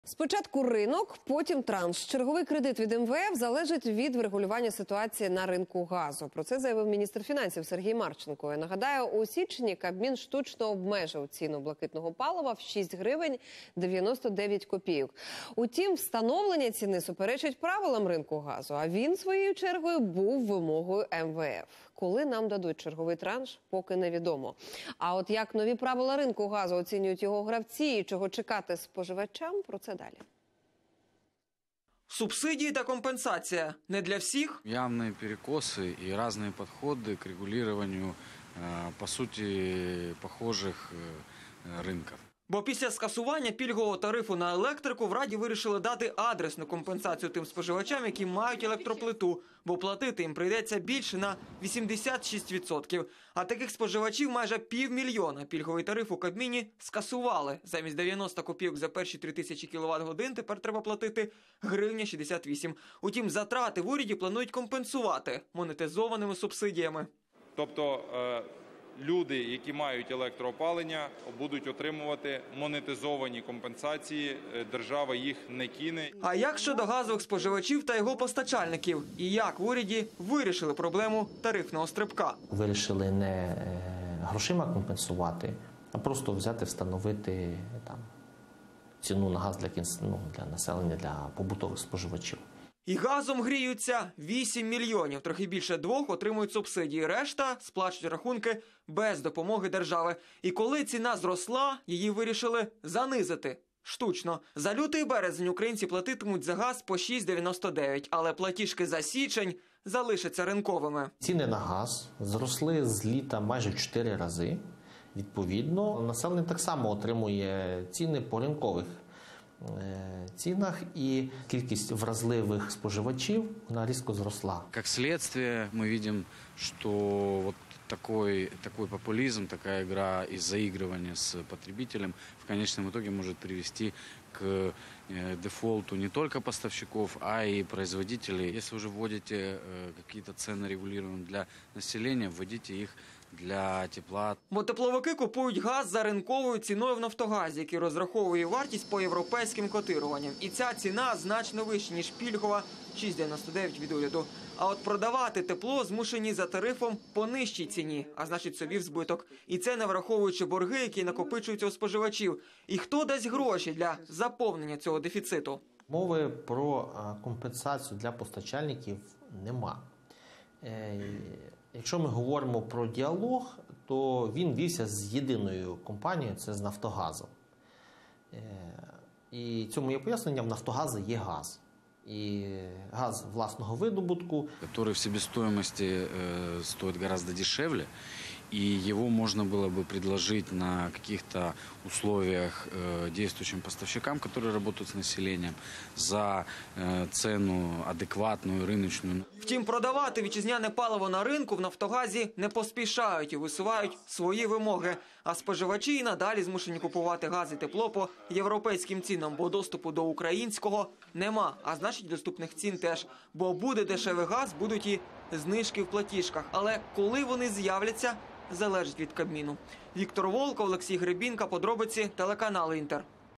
Ціна газу в Україні: як формуються тарифи для населення
Чому у "Нафтогазі" не поспішають продавати вітчизняне паливо на ринку, а споживачі й надалі змушені купувати газ і тепло за європейськими цінами?